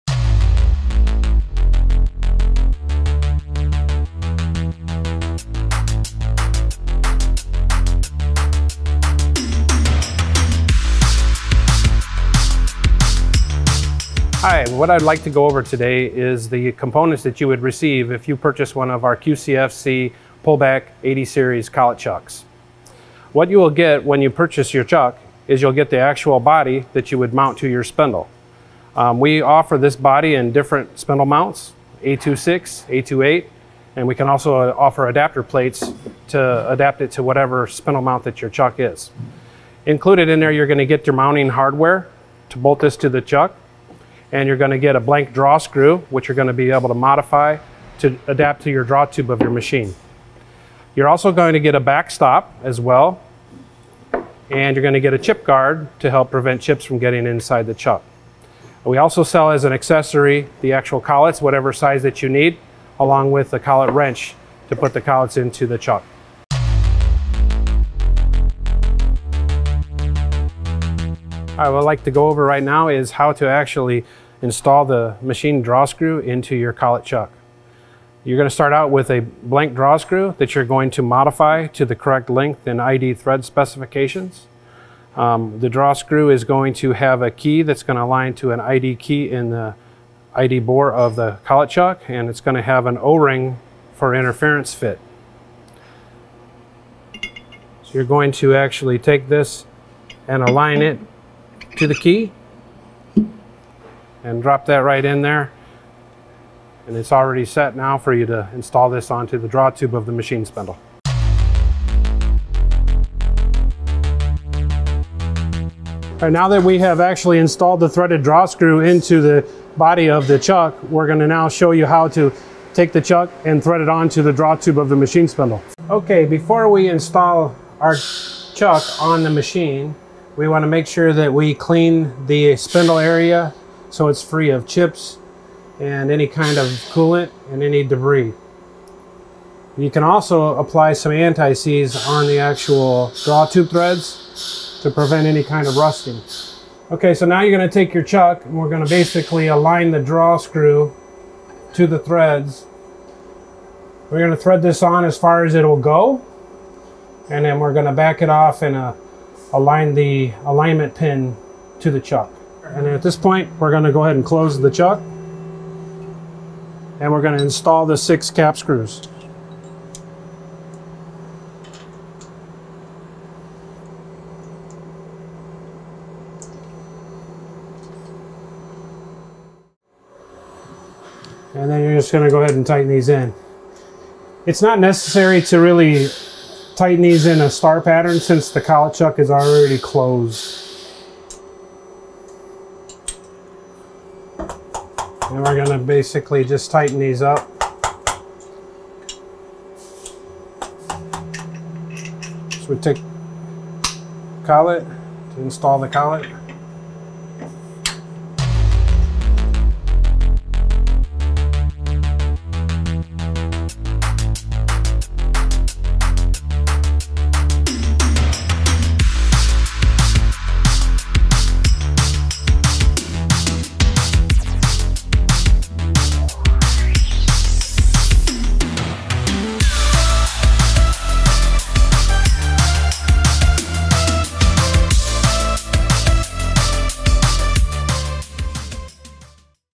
Lyndex-Nikken - HOW TO: QCFC PULL BACK COLLET CHUCKS
Learn the components of Quick Change Flex Collet (QCFC) pull back collet chuck, and see how to install the machined draw screw into the collet chuck, how to thread the chuck onto the draw tube of the machine spindle, and how to check run out.

Lyndex-Nikken provides high performance machine tool accessories recognized worldwide for their amazing precision and reliability.